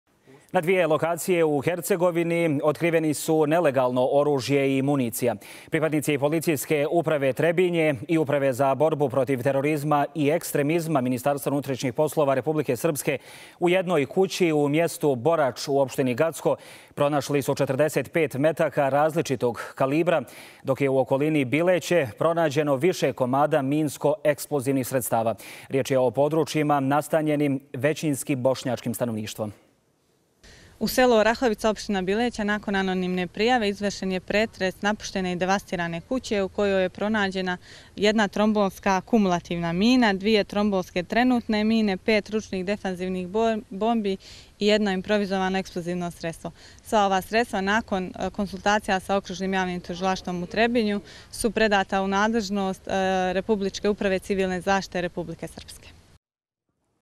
Antiteroristička akcija MUP-a Srpske u selu Orahovica kod Bileće
Pripadnici PU Trebinje i Uprave za borbu protiv terorizma i ekstremizma MUP-a Republike Srpske, nastavljajući aktivnosti na sprečavanju nelegalnog prometa i posjedovanja oružja i minsko-eksplozivnih sredstava pretresli su kuće i pomoćne objekte na dvije lokacije u Gacku i Bileći.